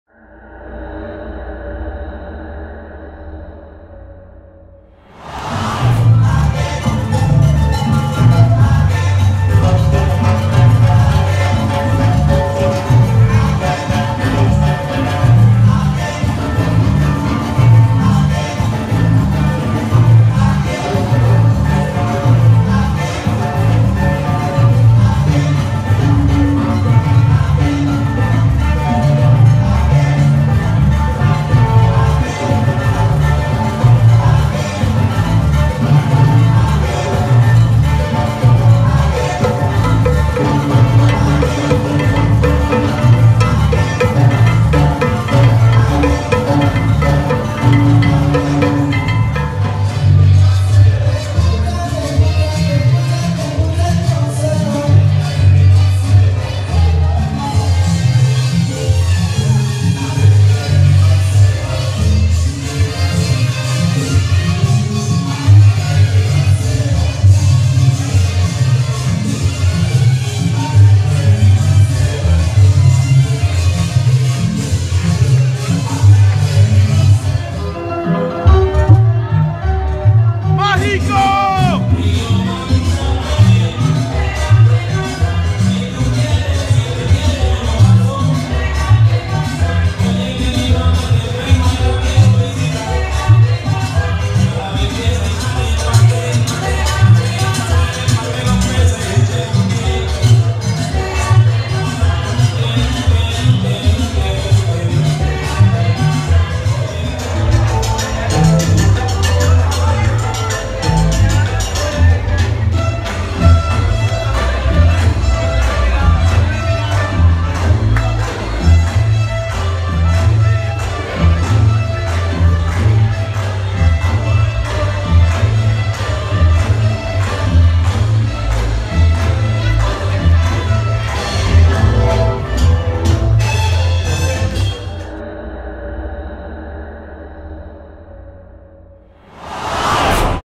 Los Angeles Bachata festival 2015 - Mambo OnWax
Salsa & Mambo room hosted be Mambo  OnWax